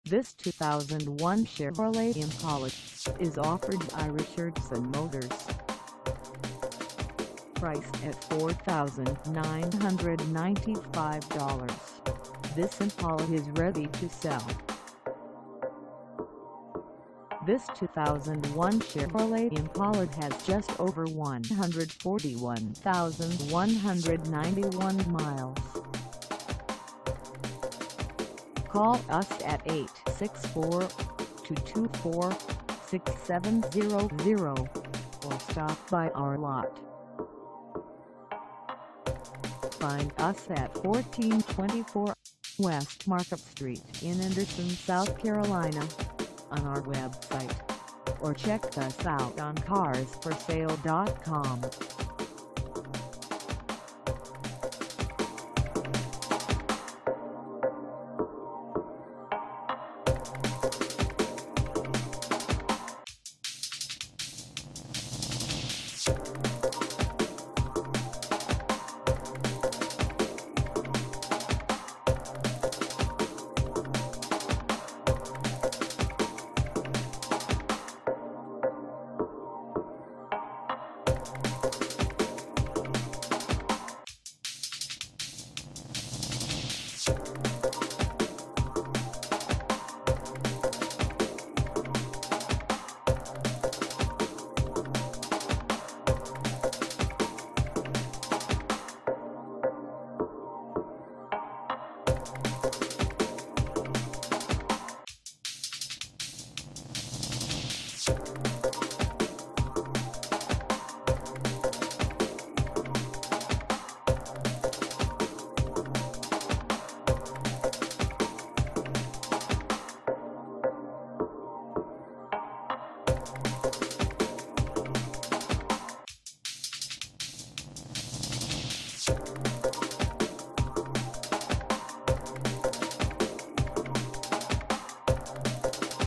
2001 Chevrolet Impala Base - for sale in ANDERSON, SC 29624
RICHARDSON MOTORS
1424 W MARKET ST

Come test drive this 2001 Chevrolet Impala Base for sale in ANDERSON, SC.

 This is a super nice Chevy Impala car runs and drives perfect. We have completely serviced this vehicle gave her everything she needed and now she is ready to go. Come by and test drive her or call Bo or Wayne .  There are no electrical problems with this vehicle.  No defects.  We have found no door dings on this vehicle.  This vehicle is in good running condition.  The exterior finish on this automobile is in excellent condition.  It is obvious that this vehicle has been garaged.  The previous owner was a non-smoker.  The interior is in fantastic condition.  Actual miles.  Great, brand hammer new smooth shifting transmission.  There is no damage present on this vehicle.  New tires.